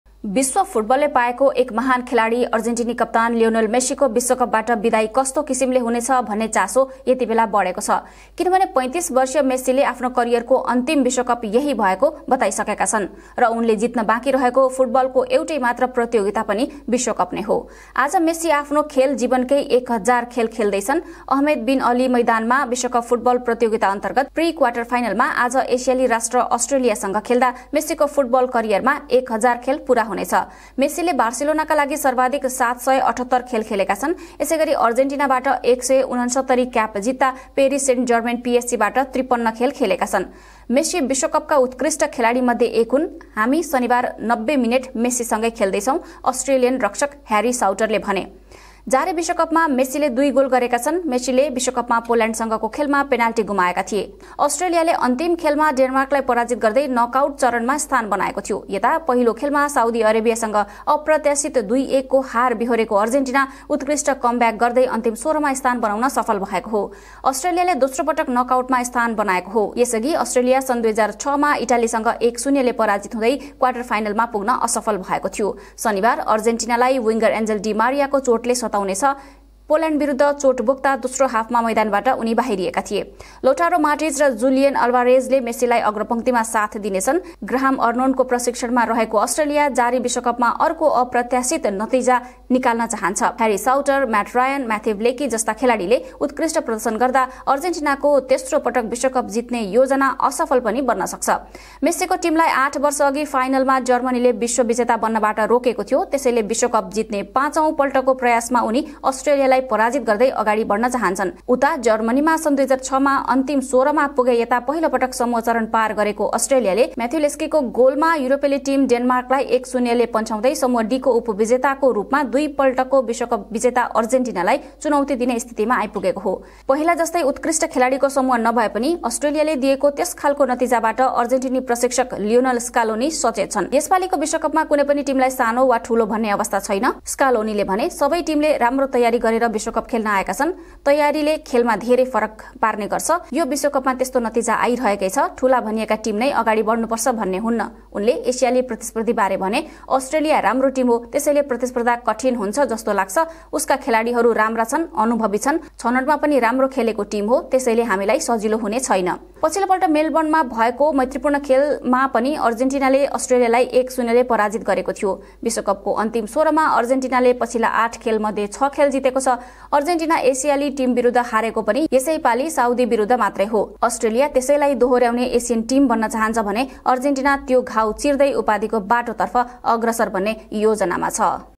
अस्ट्रेलियासंग खेल्न सजिलो हुने छैनः अर्जेन्टिनी प्रशिक्षक || Nepal Times
अस्ट्रेलियासंग खेल्न सजिलो हुने छैनः अर्जेन्टिनी प्रशिक्षक

Produced By: Nepal Times Media Private Limited
News Edited By: Harihar Nepal
Presented By: Ranjita Pokharel
Video Edited By : Nepal Times